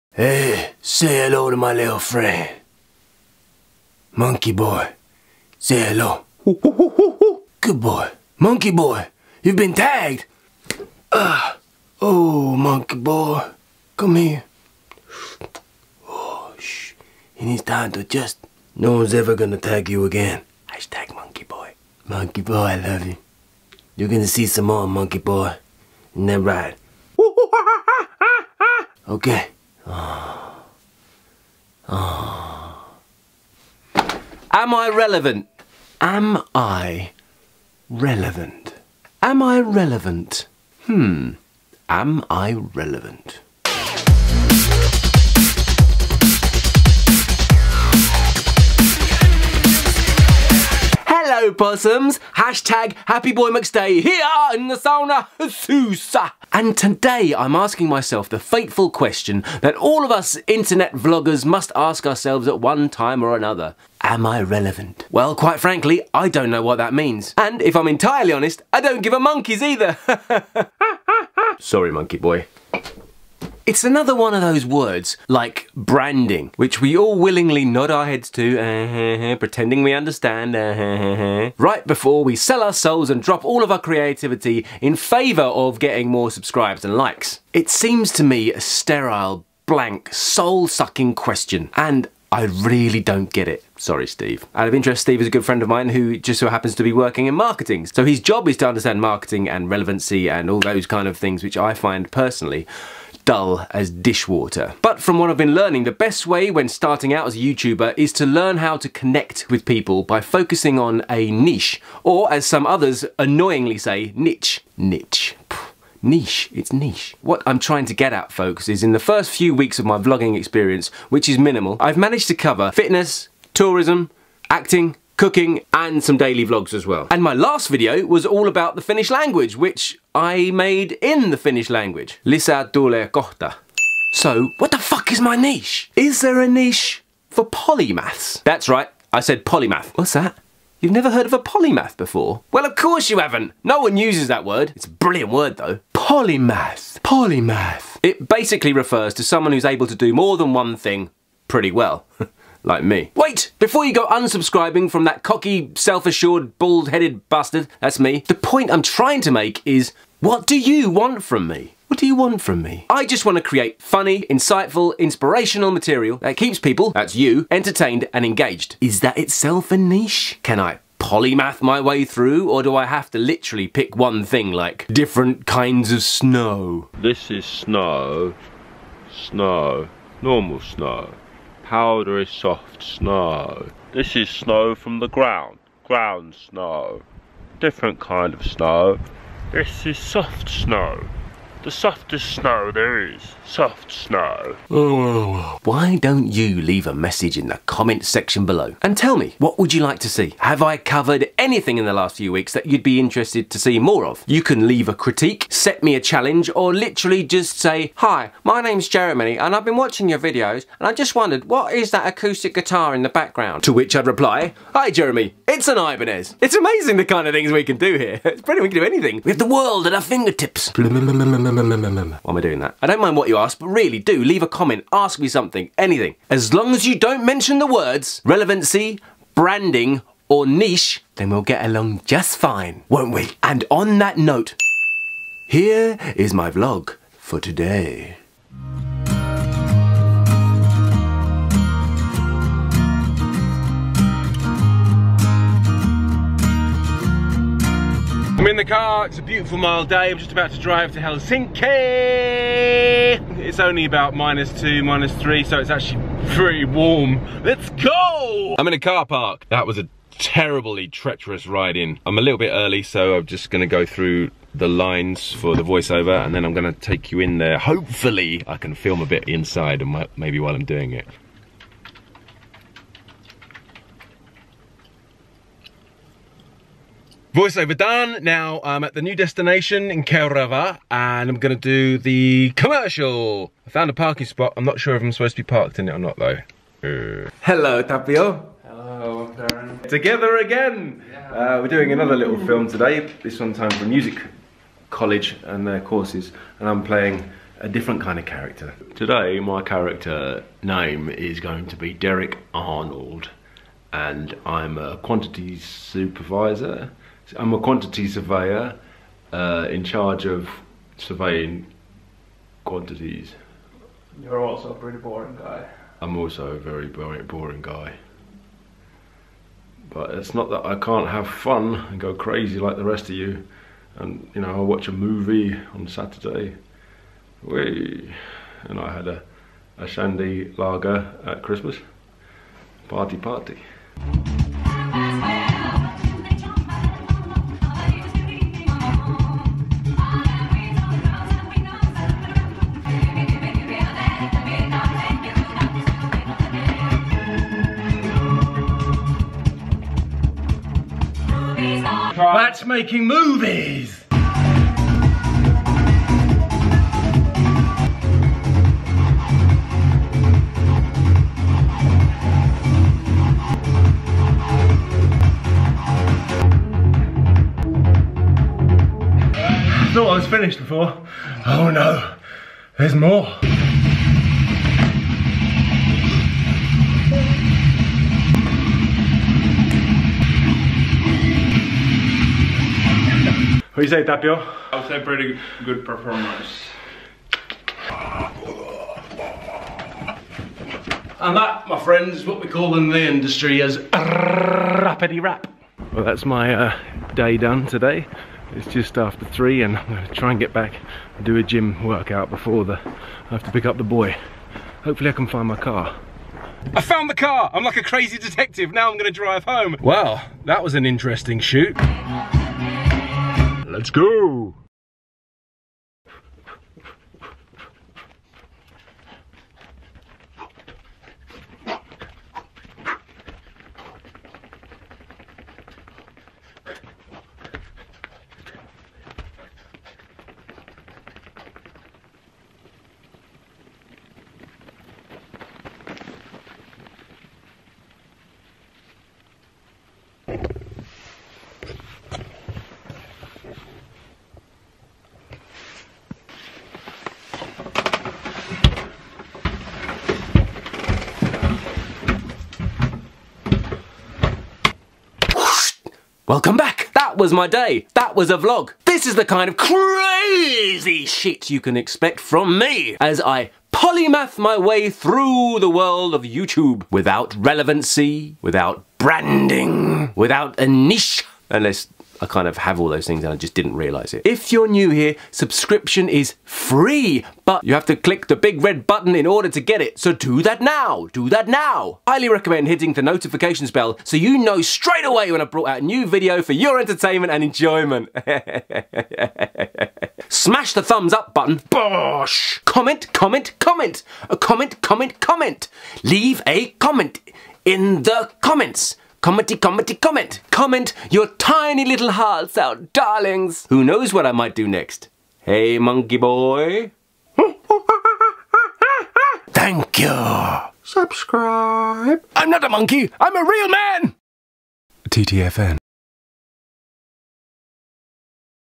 WTF Is Being Relevant?
Relevancy, Branding And Finding Your Niche - IN MY HUMBLE OPINION

WHAT IS IT? WHY IS IT? I don't know or really even care. I just want to make videos that entertain and inspire.

Much love
Darren